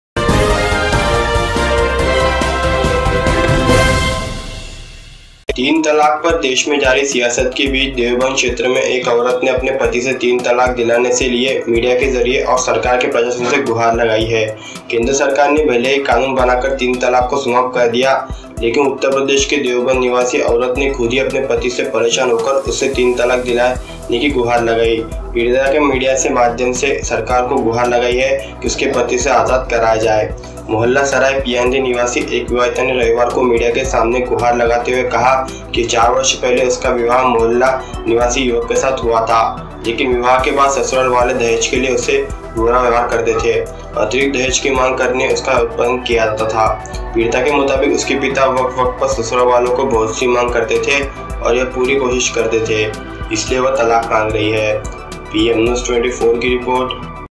3 तलाक ये औरत खुद ही अपने पति से मांग रही है , जानिये क्या है कारण
3 तलाक ये औरत खुद ही अपने पति से मांग रही है , जानिये क्या है कारण

3 Divorce, this woman herself is seeking her husband, know what is the reason

Between the state of affairs released on three divorces, a woman in Deoband area has apologized to the government and administration through media to get three divorces from her husband. The Central Government may be trying to abolish triple talaq by enacting a law, but the woman resident of Deoband in Uttar Pradesh herself is upset with her husband and has requested him to get triple talaq. The victim has pleaded with the government through the media that she should be freed from her husband.

A married couple resident of Mohalla Sara Peerzadgan, on Sunday, said through media, that her marriage was done four years ago with the young man resident of Pathanpura. But since the wedding, the in laws have behaved badly with dowry. They have been harassing him by demanding additional dowry. According to the victim, her father also tried to meet many of the in-laws' demands from time to time.